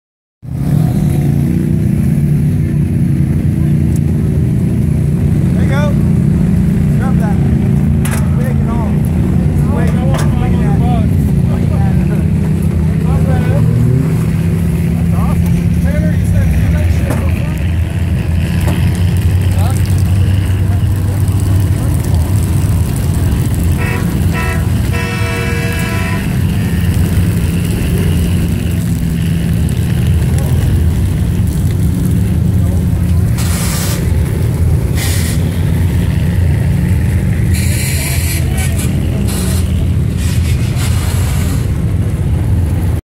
Rednecks with Paychecks 2016 winners of the Chug Run.
Winner's of the Chug Run @RWP 2016.
Tanner and Chris you kicked ass. .
Enjoy the video and share if you like. .
Cheers